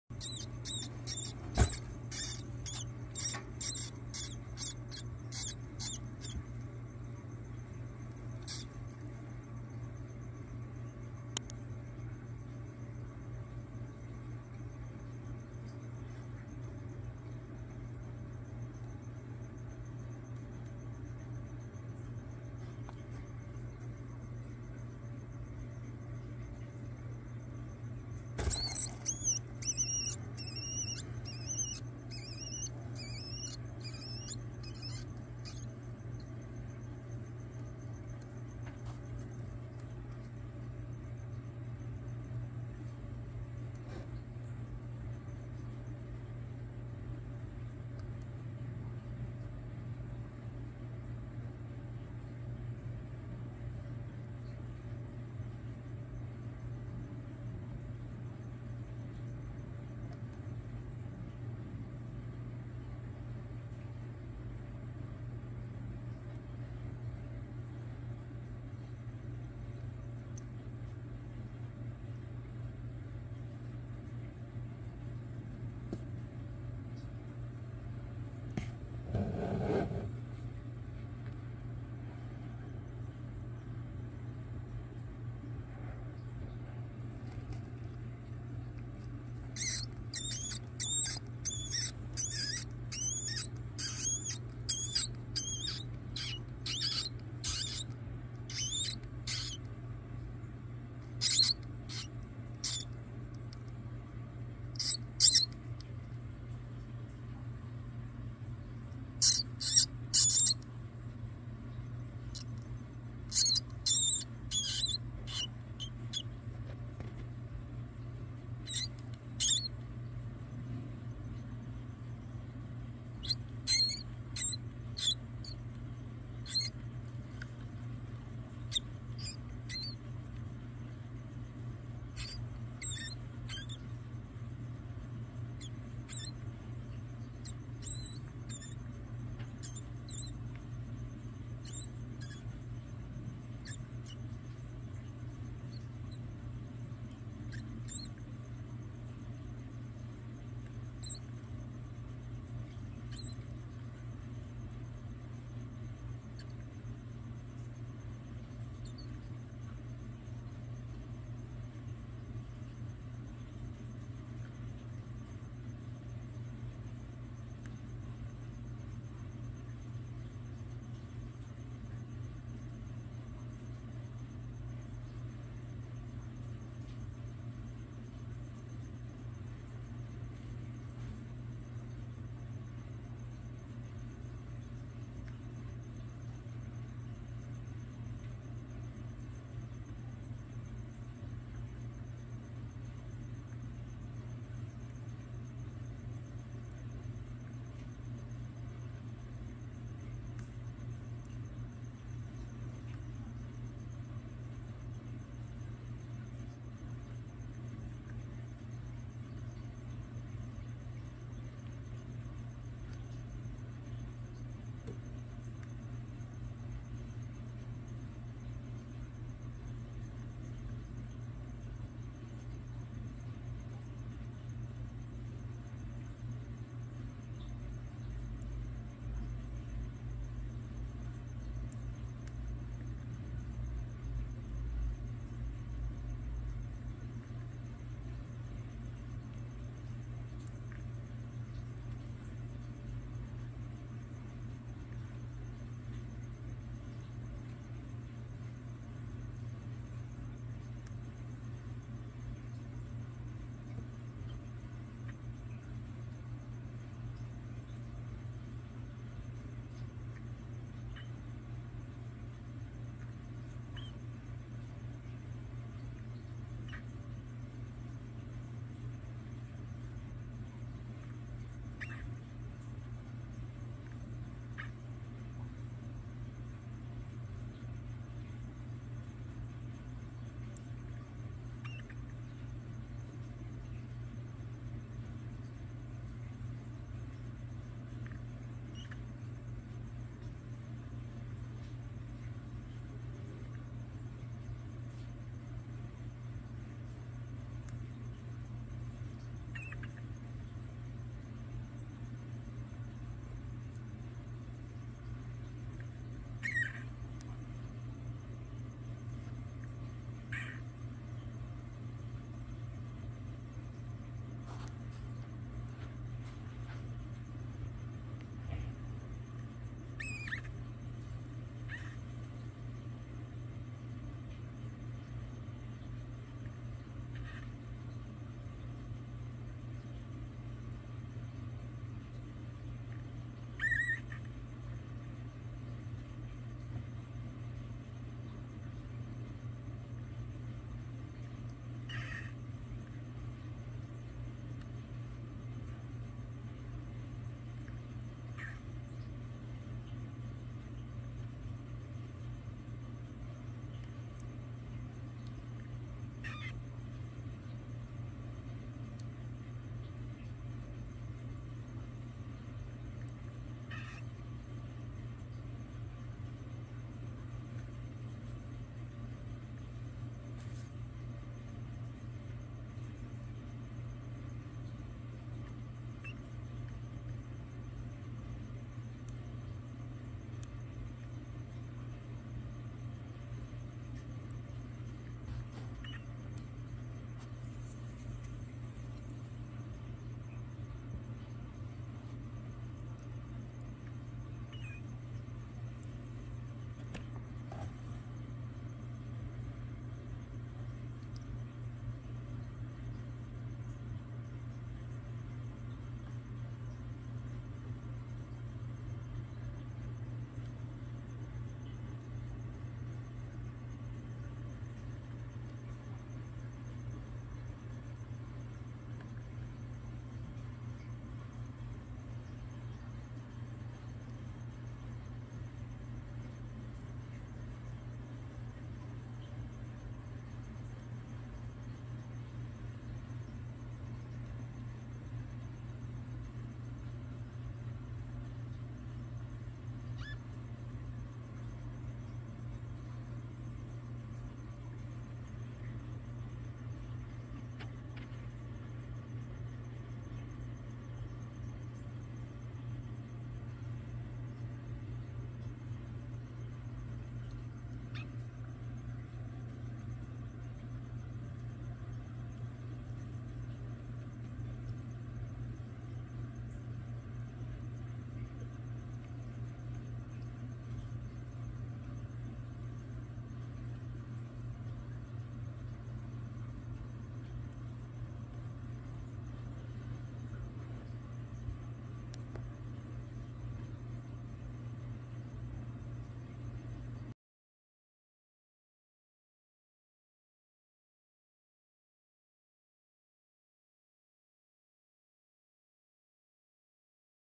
Calico Python Eats A Rat / Warning Live Feeding
in this video we document a ball python eating a rat .